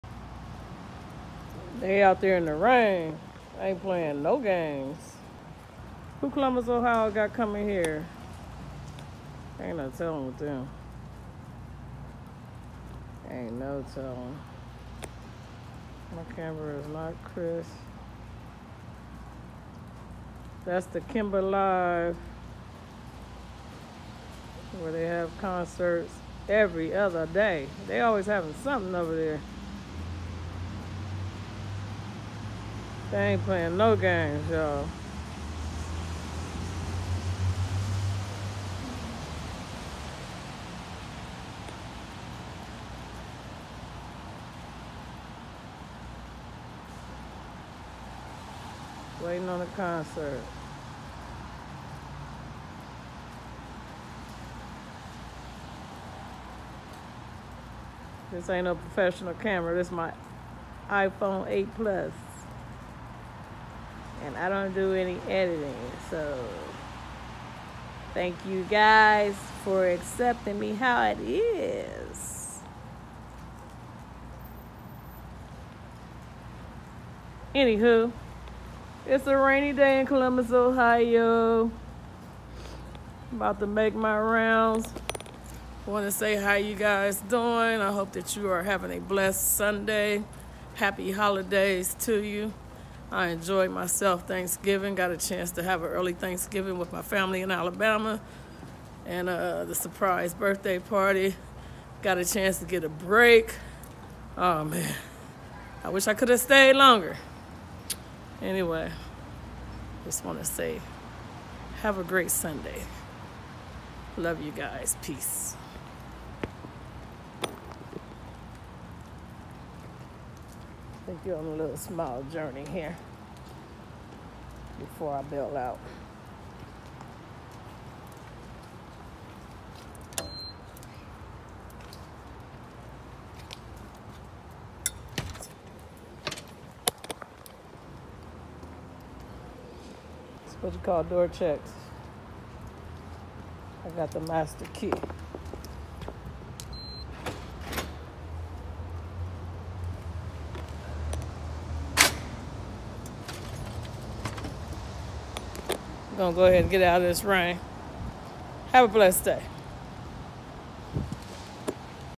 Rainy ☔️ Sunday Afternoon In Columbus Ohio
Just Saying Hello And Appreciating Life Even When It Rains 😎